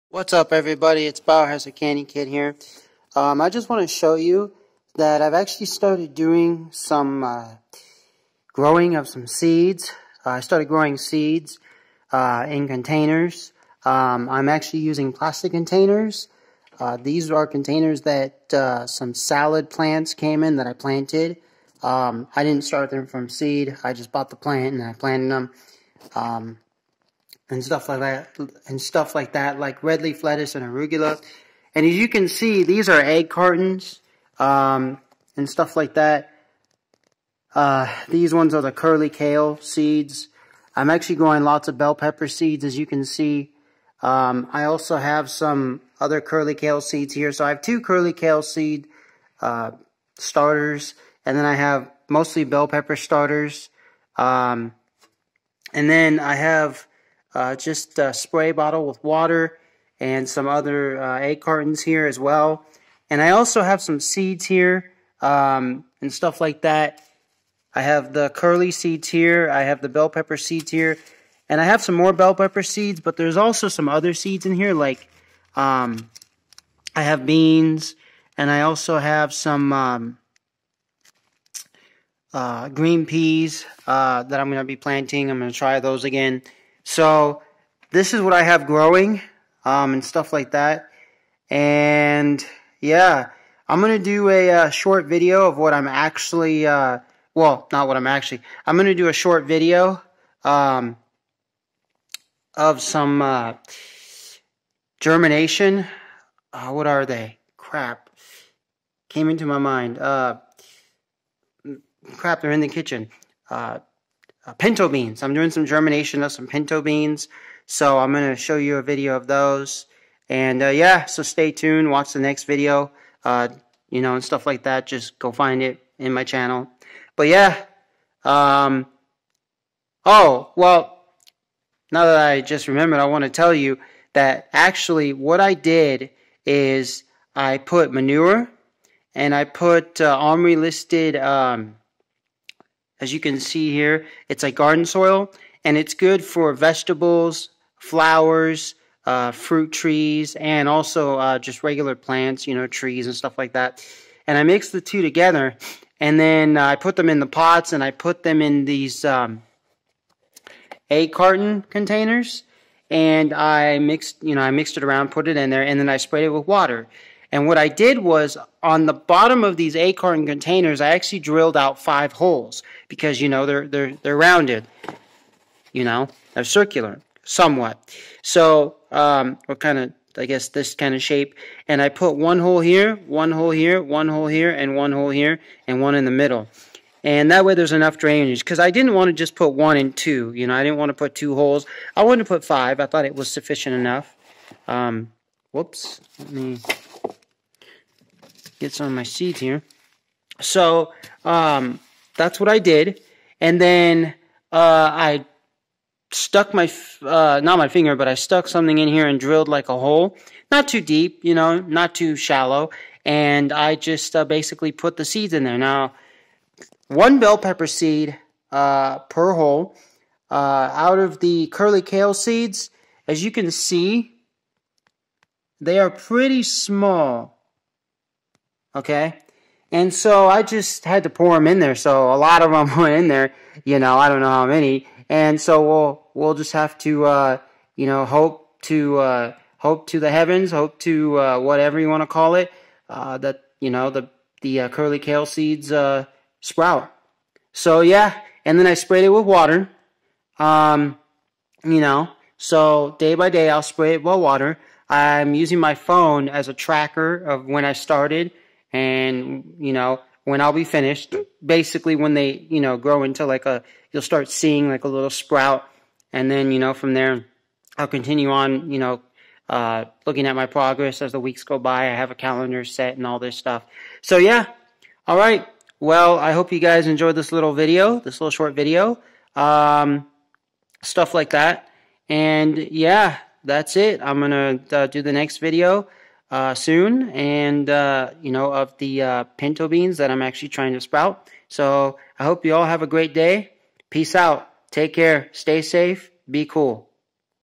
Starting plants from seeds in starter containers
I hope you enjoy this short video I made on seeds in starter containers I just started doing these it's not my first time but I hope this will not be my last either because of this time is successful then I will continue doing this more and more take care stay fresh and keep on growing.